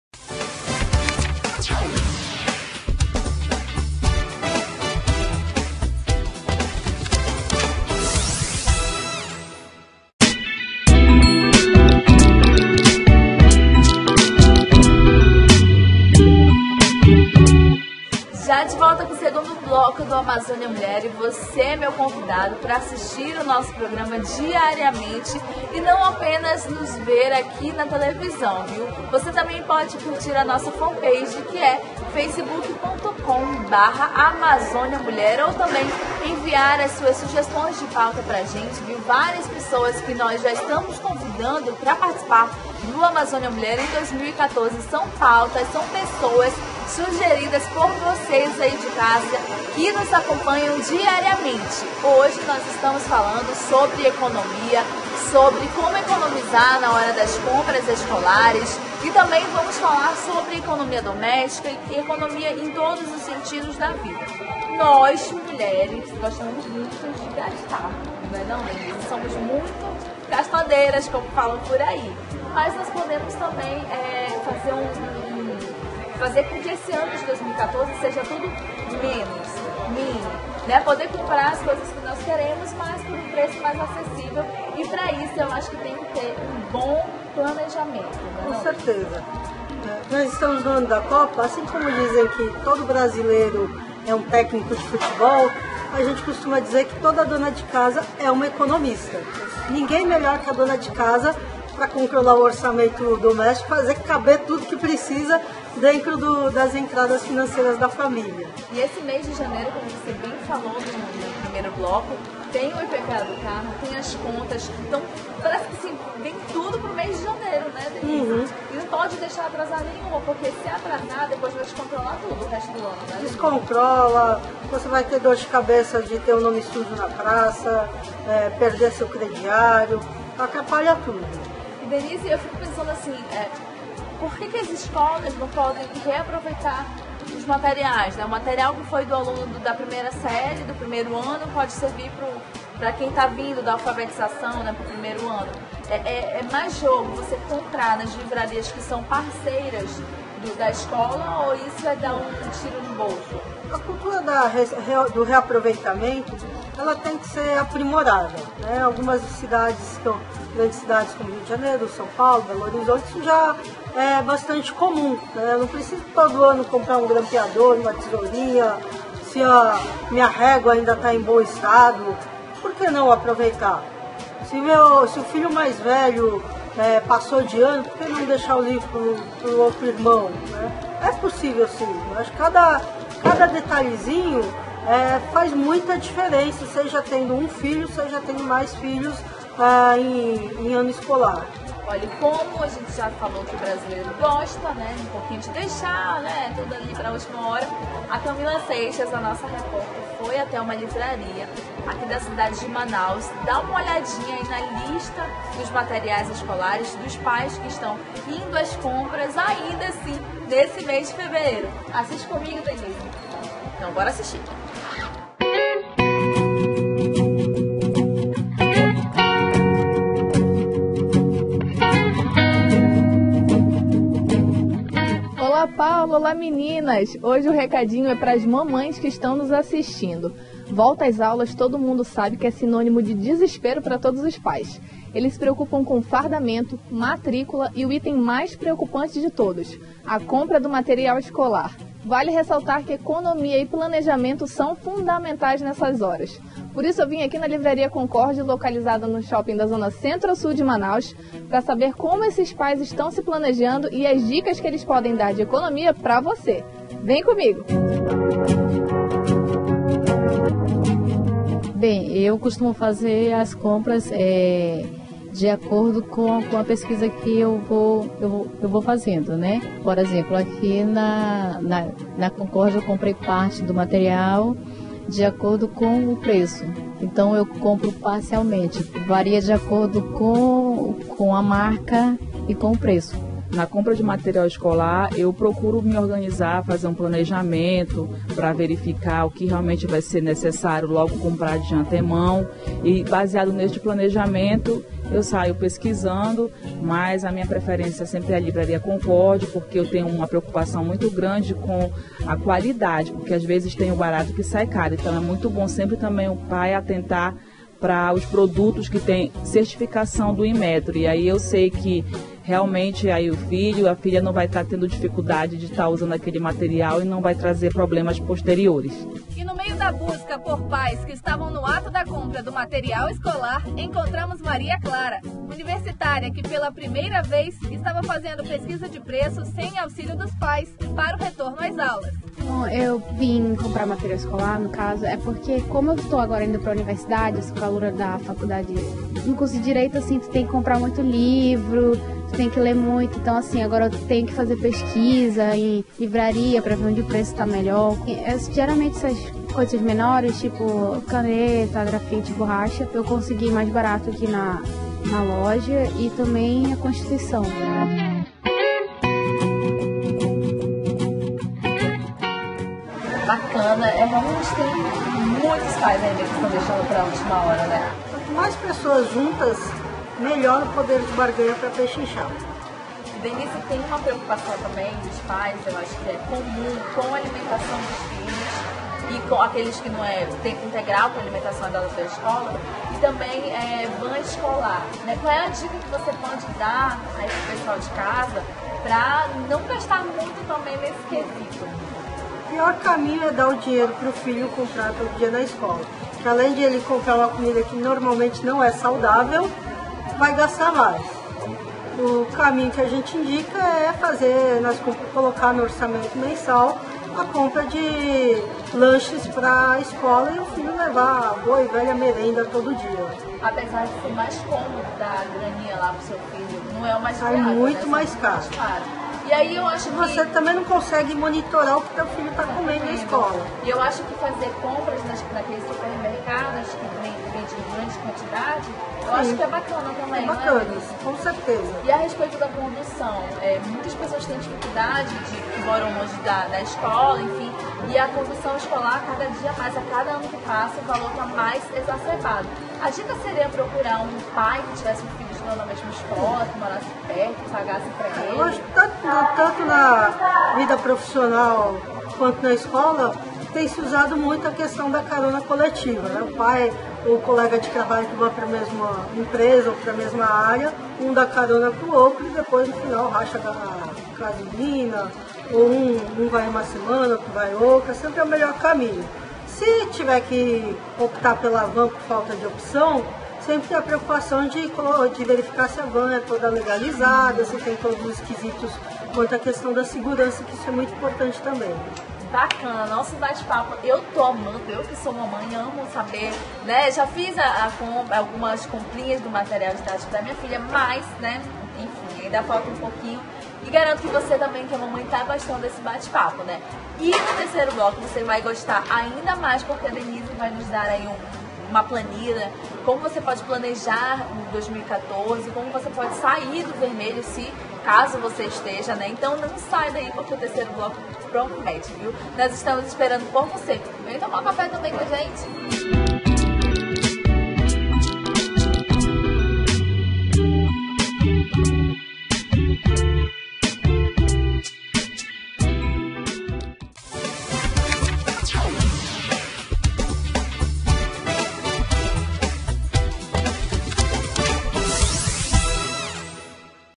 Dicas de economia e planejamento por Denise Kassama
Denise Kassama ensina como planejar na hora das comprar e evitar problemas posteriores com gastos desnecessários. Confira também matéria feita com pais que deixaram as compras pra última hora e as dicas que eles tem a dizer!